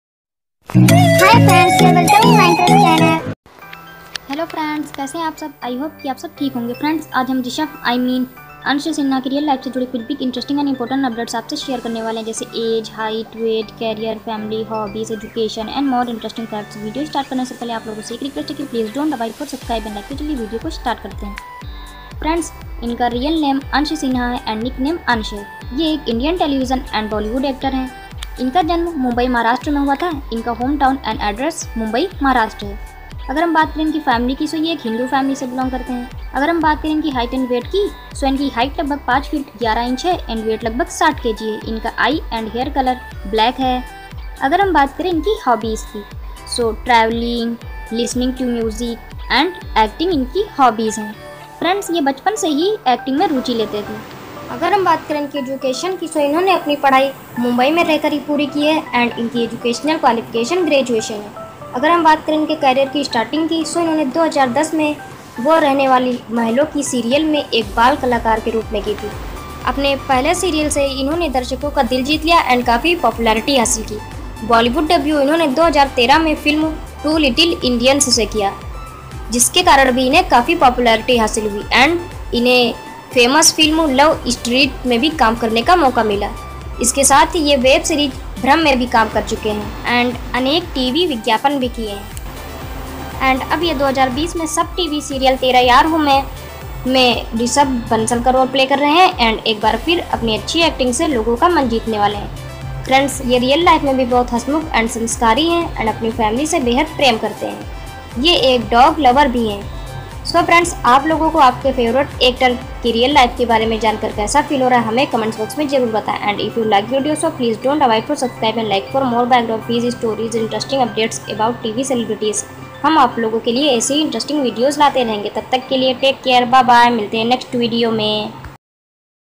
Ansh sinha, (Rishabh) Lifestyle। Family, Career, movies, serials & more।Tera Yaar hoon main,Sub TV।
Friends this is an important big and interesting updates about Ansh Sinha।as:Age, height, Wight, family, career, hobbies, education and more।